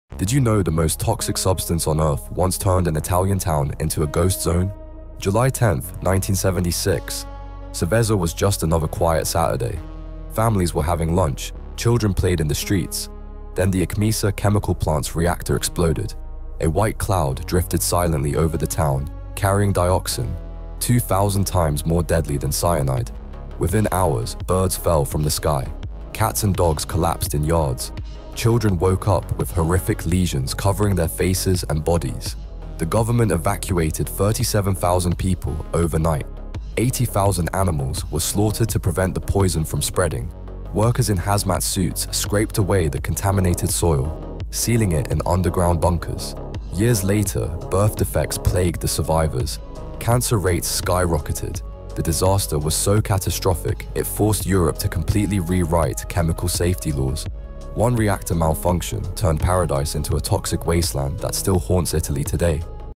The Chemical Cloud That Turned Italy Into a Ghost Town
Italy's deadliest chemical disaster 💀